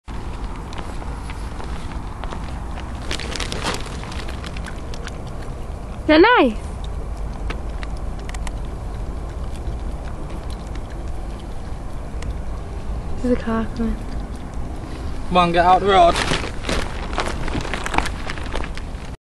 MY DOG SNOOP eating ice-cream 2010
this is my 12 week old dog snoop eating ice-cream